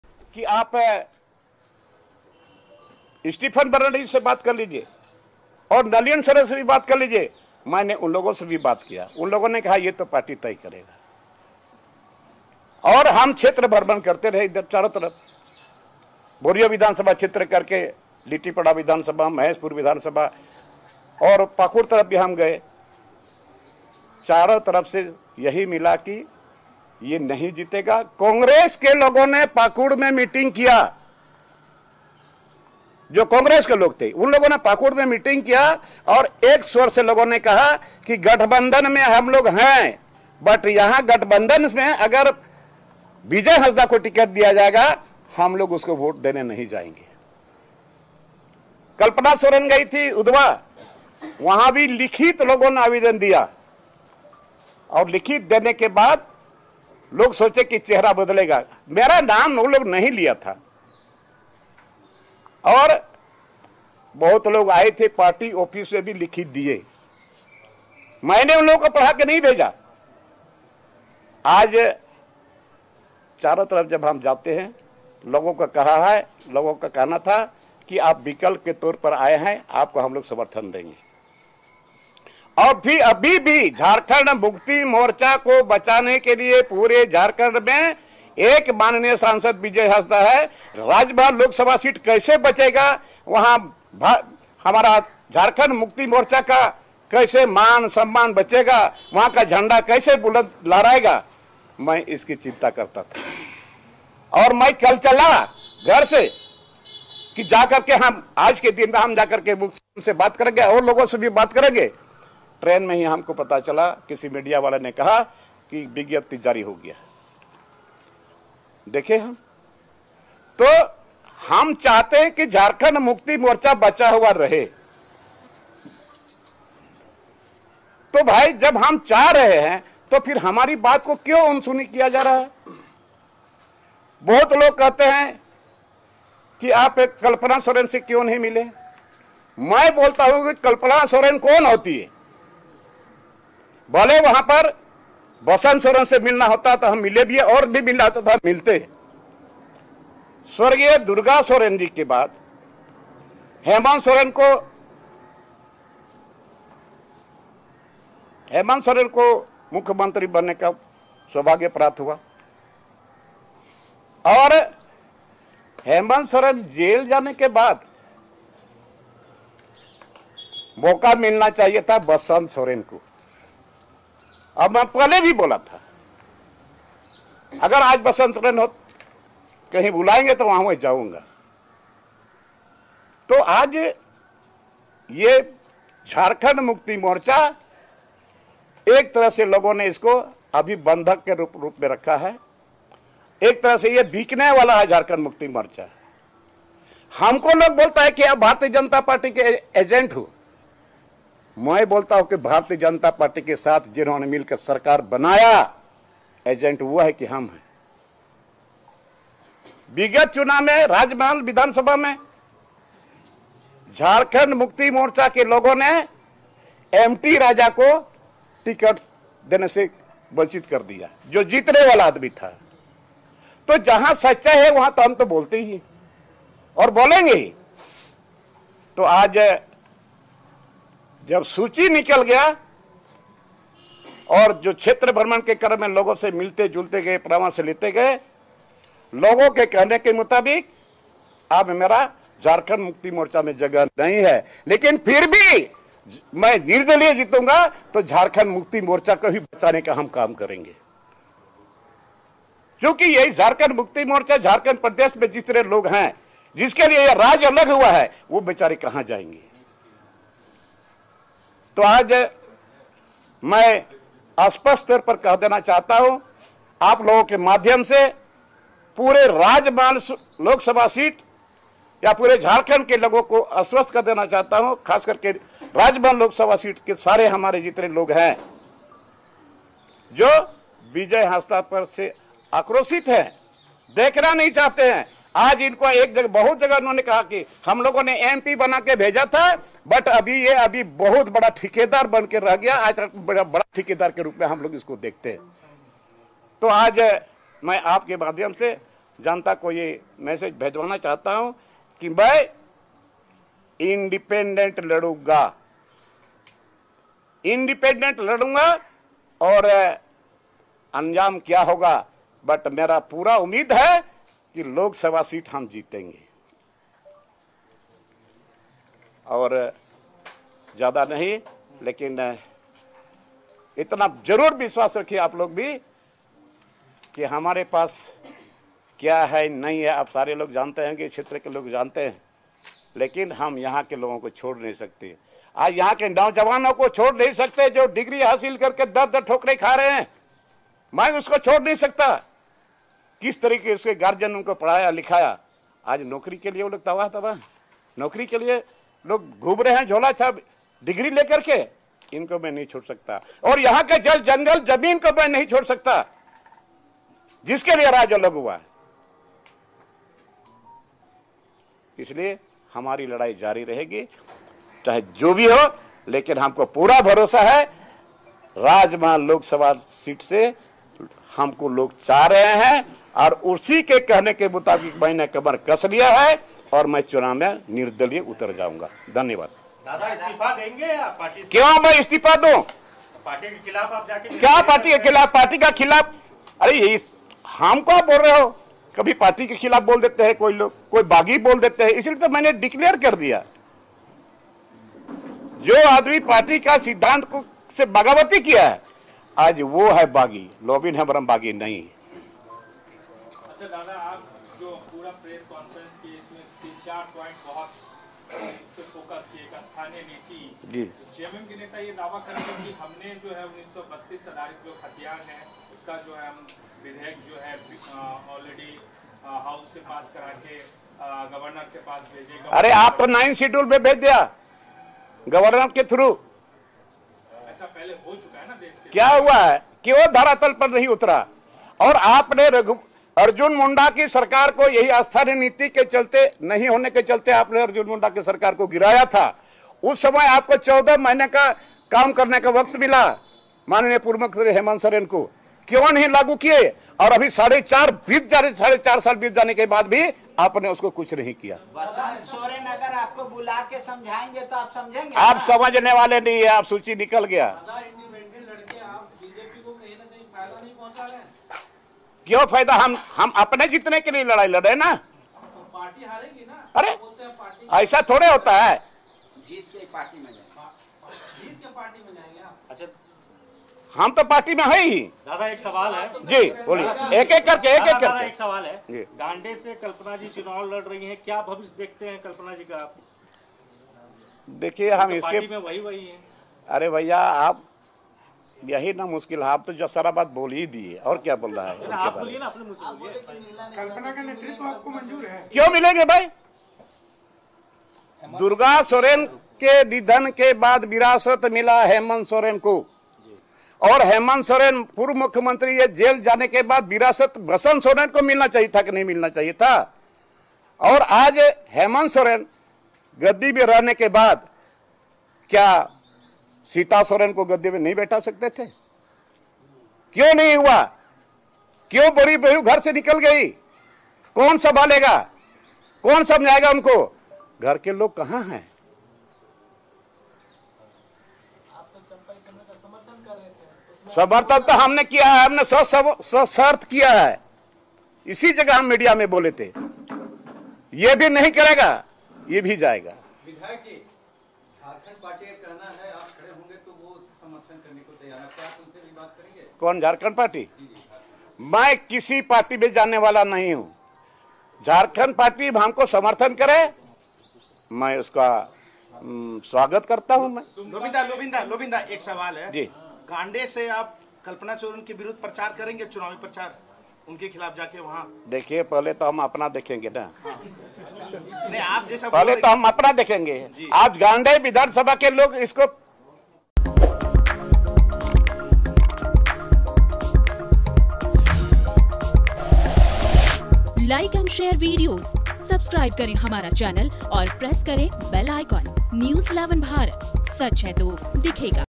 Lobin Hembrom की प्रेस कांफ्रेंस 'राजमहल से निर्दलीय लड़ेंगे चुनाव'
Lobin Hembrom की प्रेस कांफ्रेंस 'राजमहल से निर्दलीय लड़ेंगे चुनाव'jharkhand